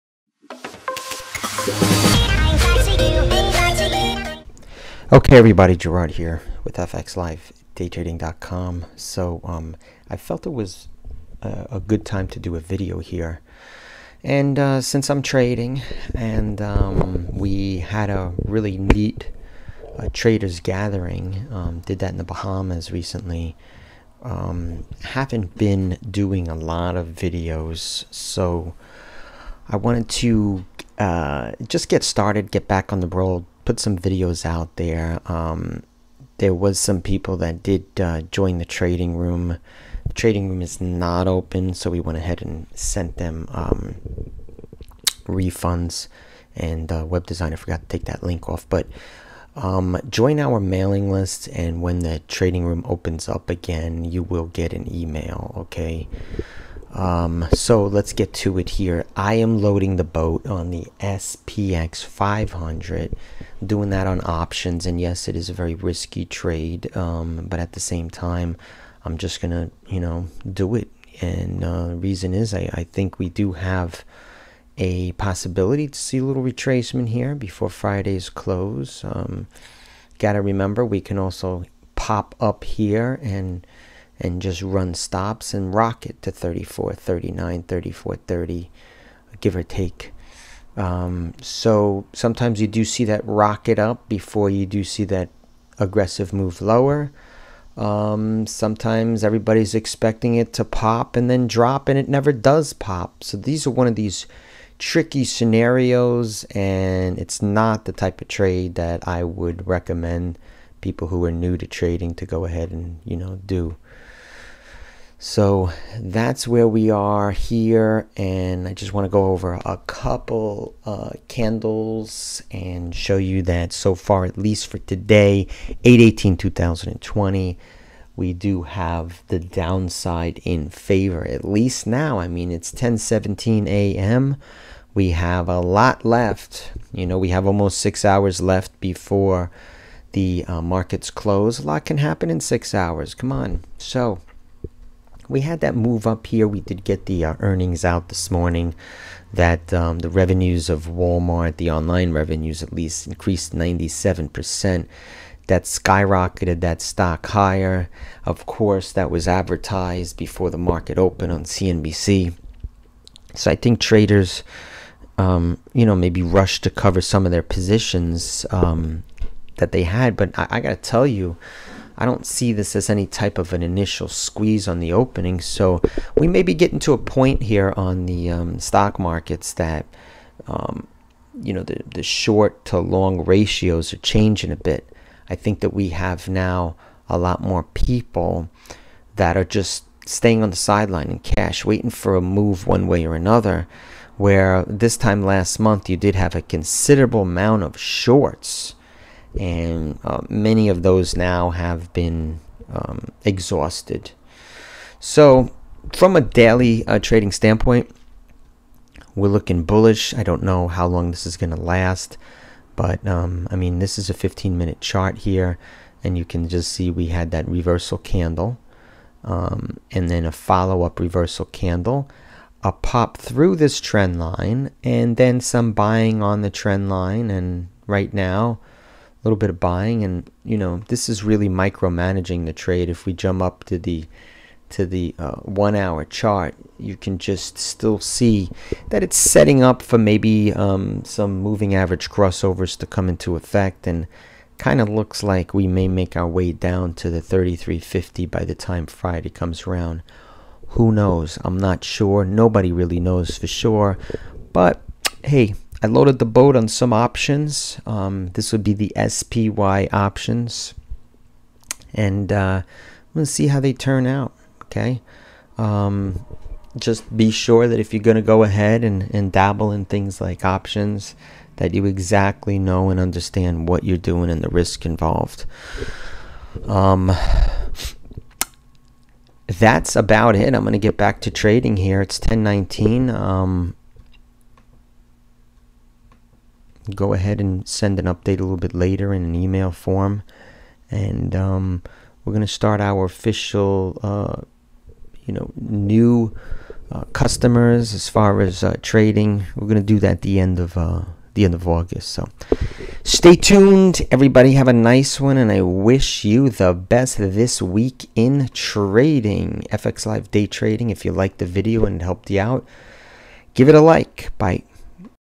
Loading up on spx500 futures options on possible double top - fx live day trading
Here is some thoughts on a possible double top on the spx500 stock trading index, we will see how this works out. Our trading room took a recess recently as we met at a traders meetup.